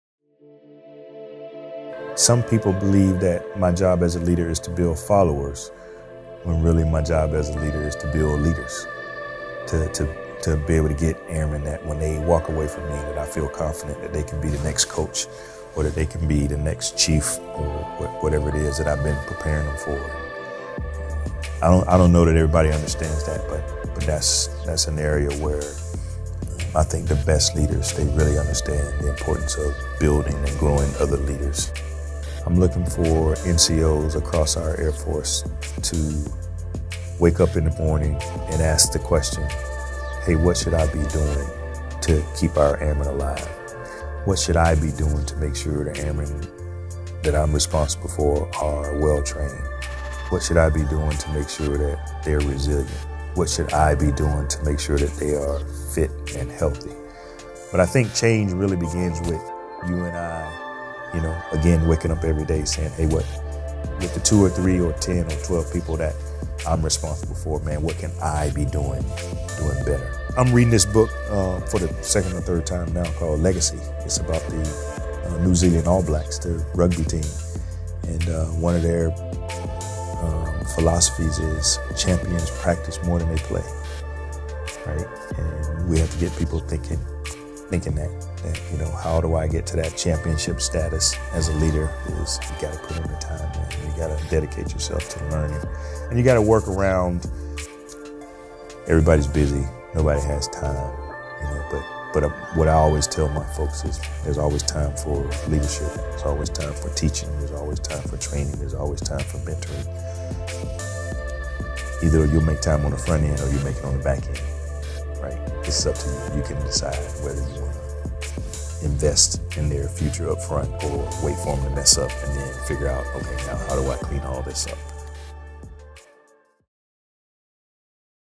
What Can I Do
Chief Master Sgt. of the Air Force Kaleth O. Wright talks about leadership.